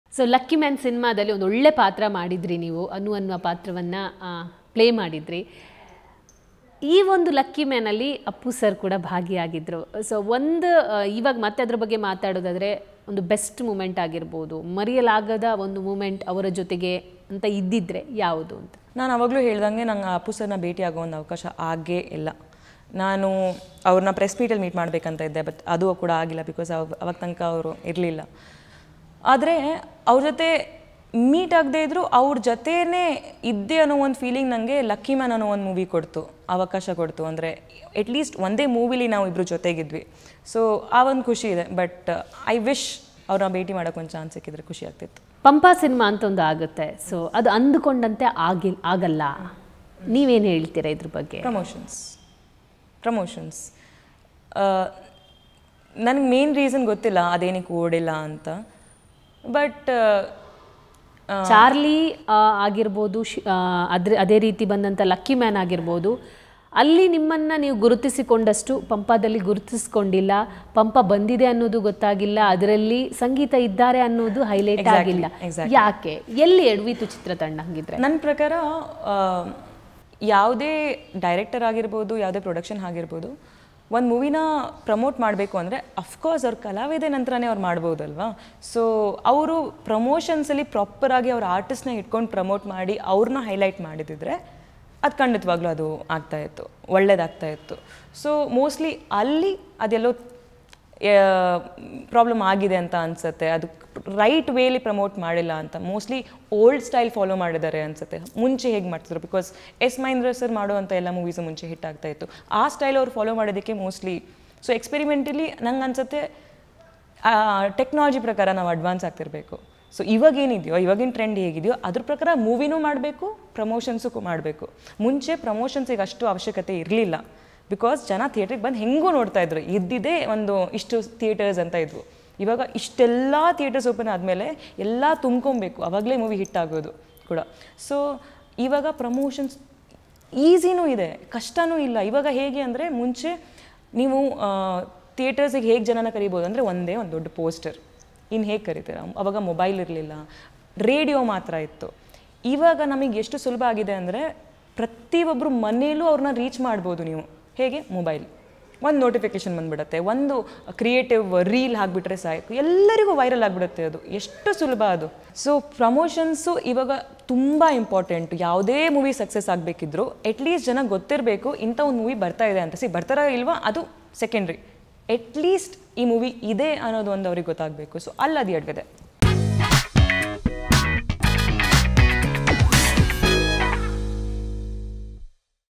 ಸಿನಿಮಾ ಸಕ್ಸಸ್ ಆಗ್ಬೇಕು ಅಂದ್ರೆ ಇರೋದು ಒಂದೇ ಮಂತ್ರ... | Sangeetha Sringeri Talk About Puneeth Rajkumar
ಚಾರ್ಲಿ 777 ಚಿತ್ರದ ನಾಯಕಿ ಸಂಗೀತಾ ಶೃಂಗೇರಿ ವಿಸ್ತಾರ ವಿಶೇಷ ಸಂದರ್ಶನದಲ್ಲಿ ಅಪ್ಪು ಮೀಟ್ ಮಾಡುವ ಅವಕಾಶ ಸಿಗದಿದ್ದ ಕುರಿತು ಬೇಸರ ವ್ಯಕ್ತಪಡಿಸಿದರು.